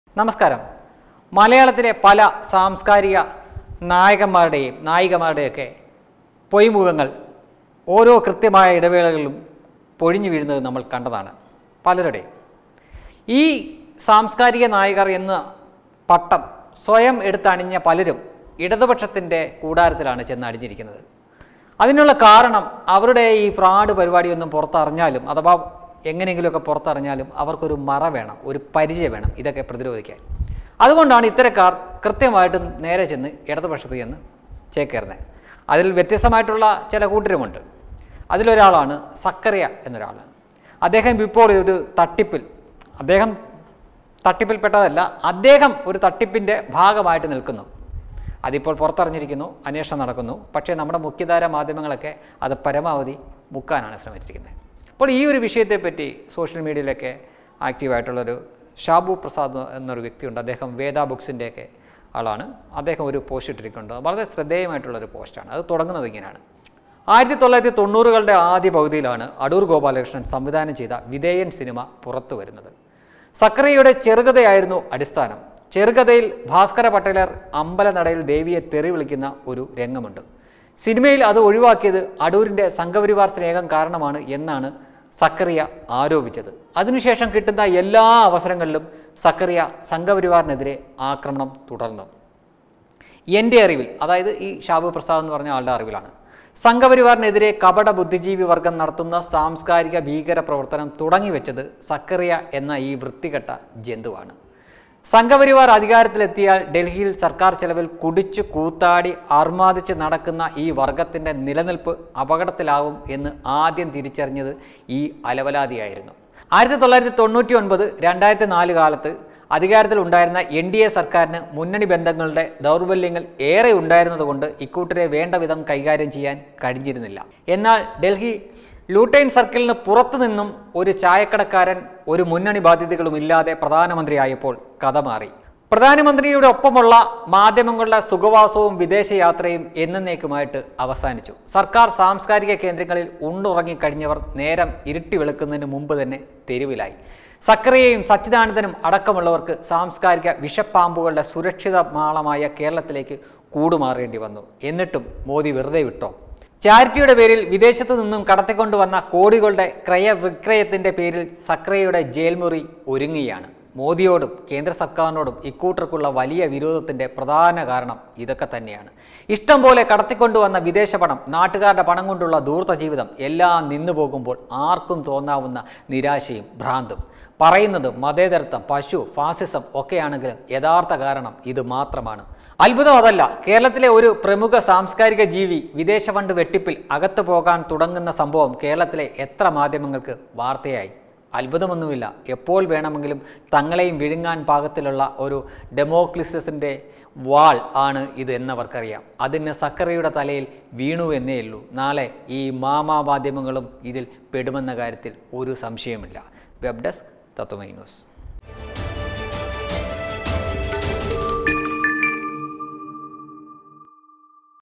എഴുത്തുകാരൻ സക്കറിയ ജയിലേക്ക് ,നടത്തിയത് വൻ തട്ടിപ്പ് | ZHACKARIA
സക്കറിയ എന്ന അലവലാതി നടത്തിയത് സാംസ്‌കാരിക ഭീകരപ്രവർത്തനമെന്ന് ഷാബുപ്രസാദ്‌ | SAKARIYA